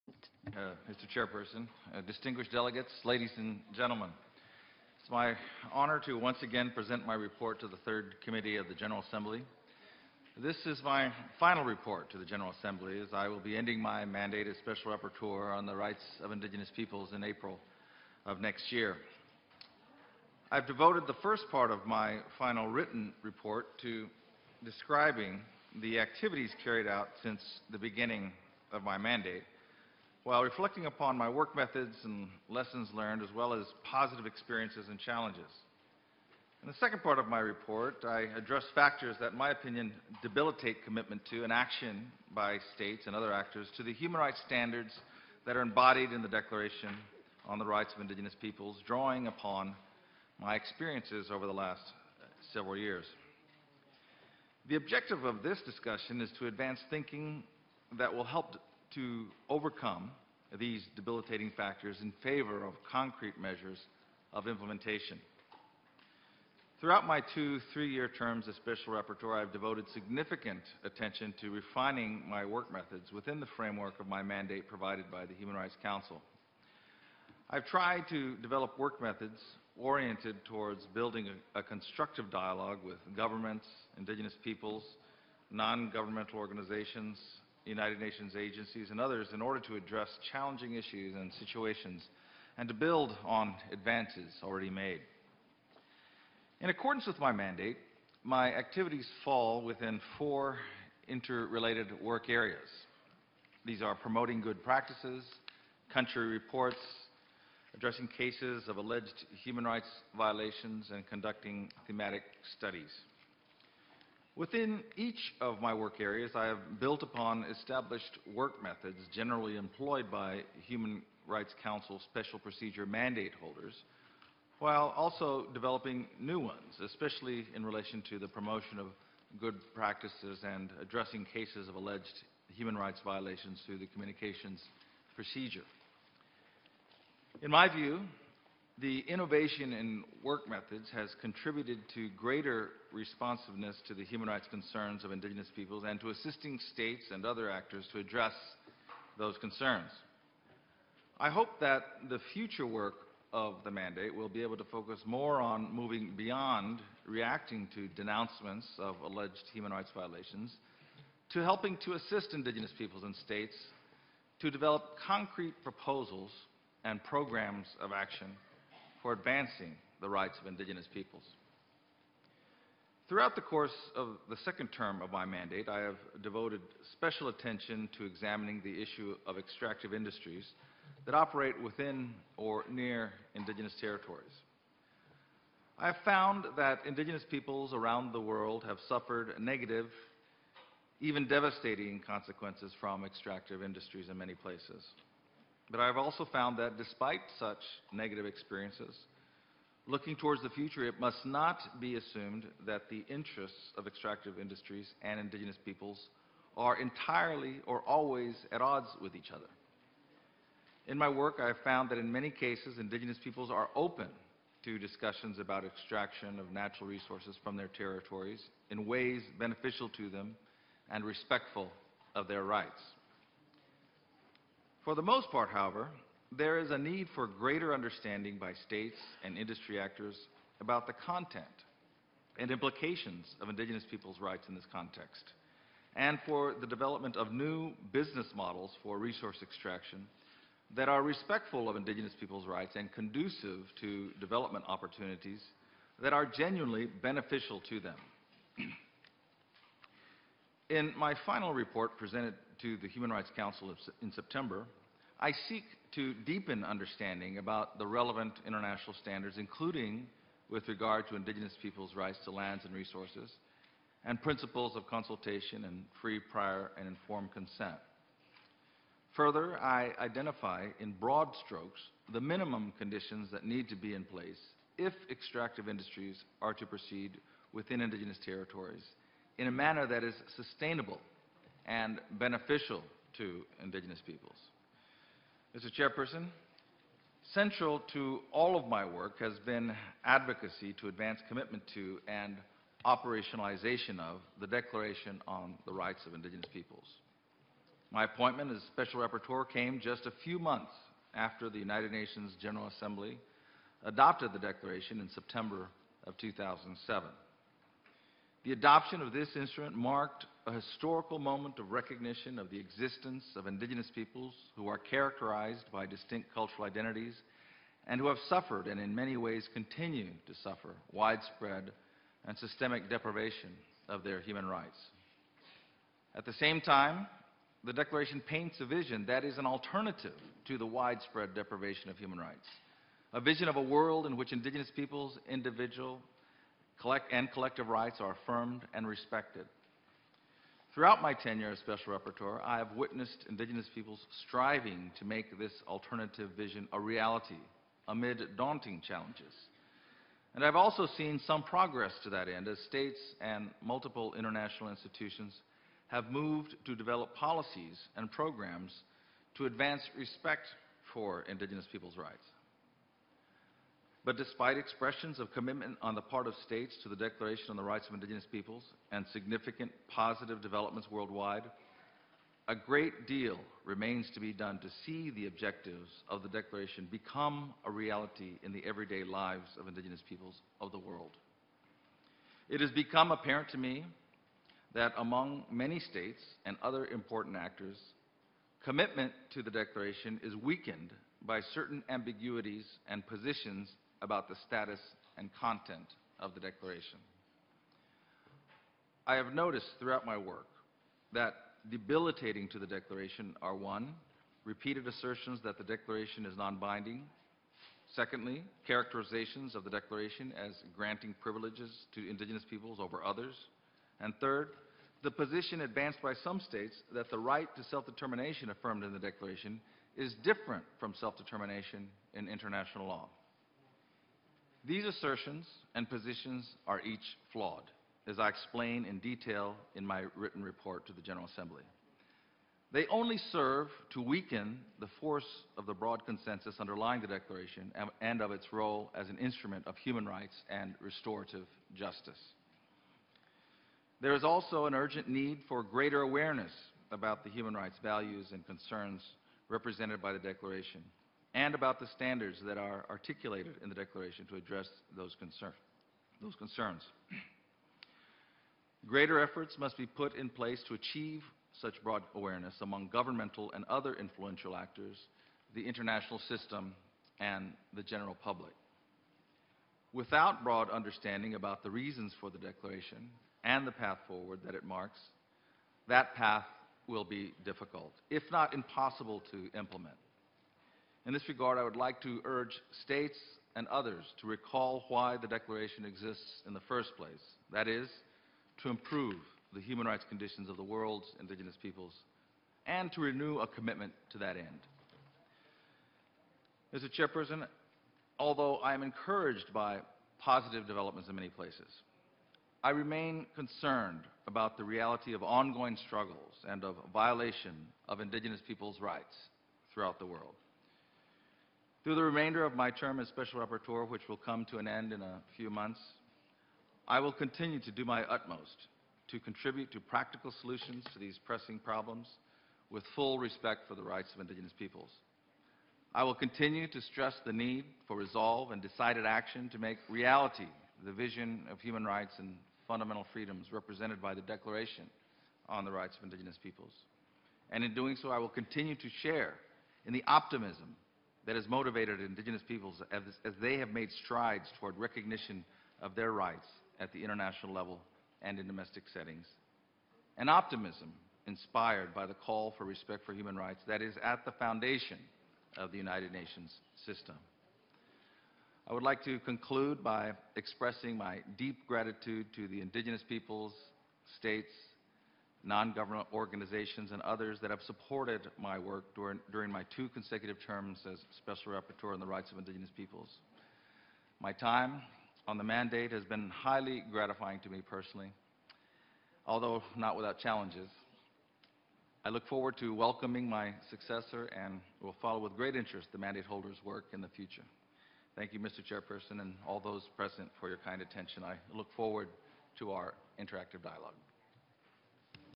James Anaya Statement to UN General Assembly, 2013
James Anaya, Special Rapporteur. Statement to UN General Assembly, 21 October 2013.

The Special Rapporteur presented his final report to the Third Committee of the United Nations General Assembly. It his statement, Professor Anaya discussed broadly his work methods and lessons learned throughout his mandate. He also touched upon factors that in his opinion debilitate commitment to and action by States and other actors to the human rights standards embodied in the Declaration on the Rights of Indigenous Peoples, drawing upon his experiences over the past years.